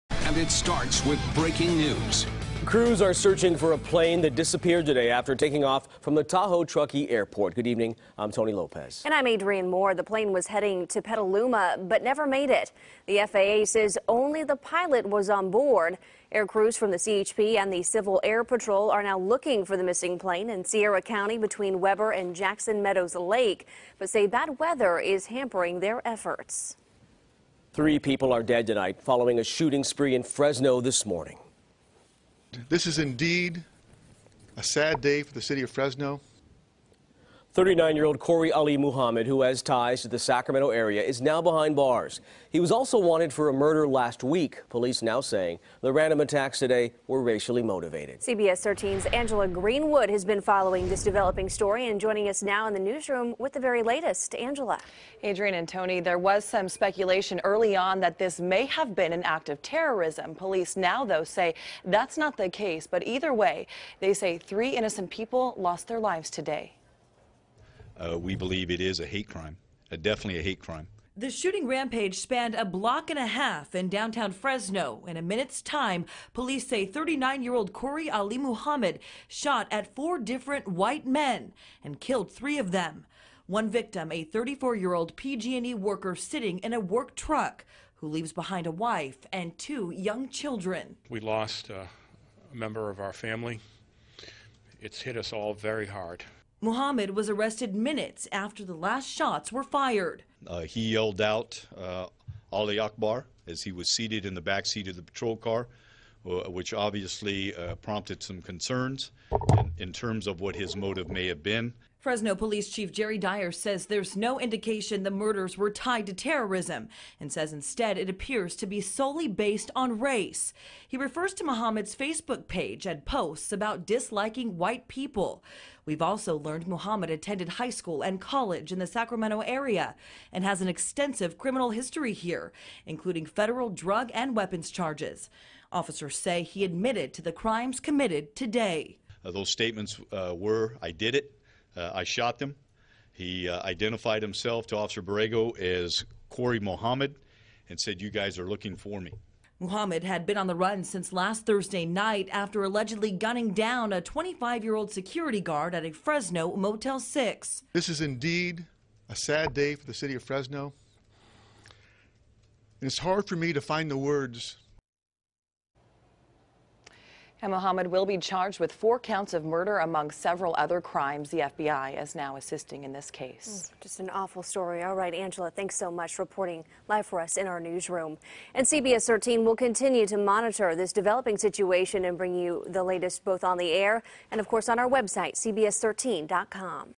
Fresno Murders Investigated As Hate Crime, Not Linked To Terrorist Group
Police say Kori Ali Muhammad, who is black, targeted white people, shooting at four of them and killing three in Fresno on Tuesday.